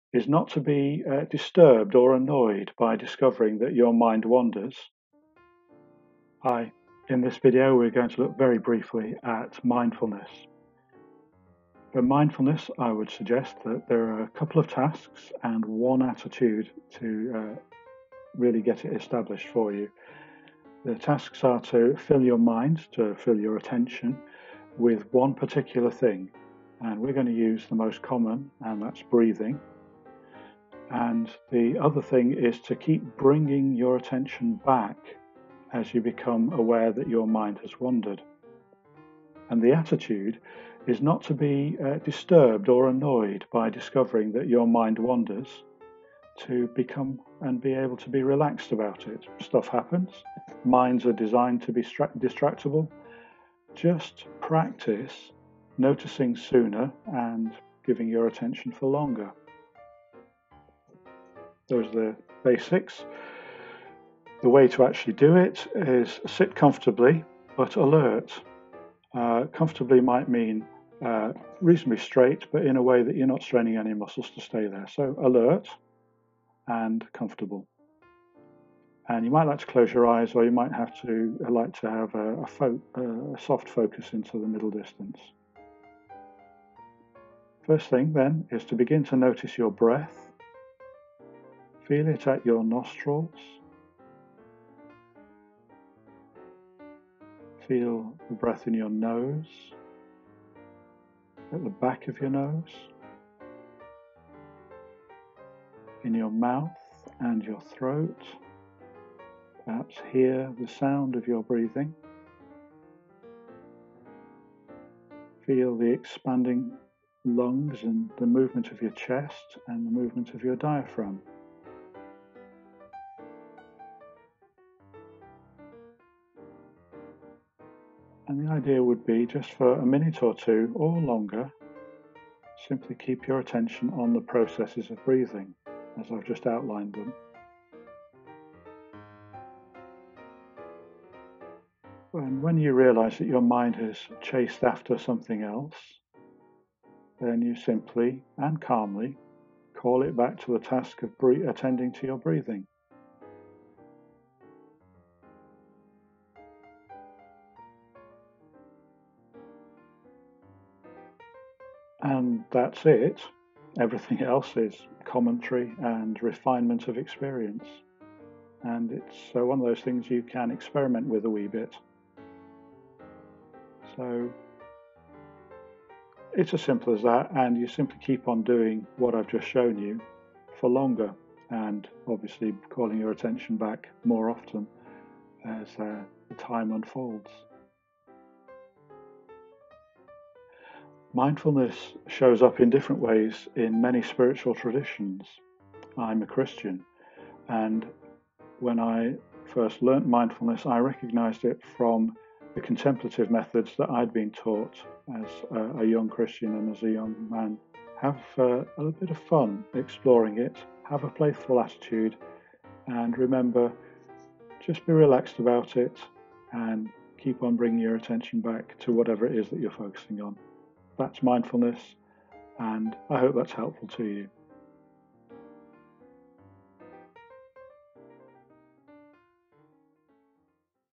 Mindfulness
Andii, university chaplain, gives us a short introduction to mindfulness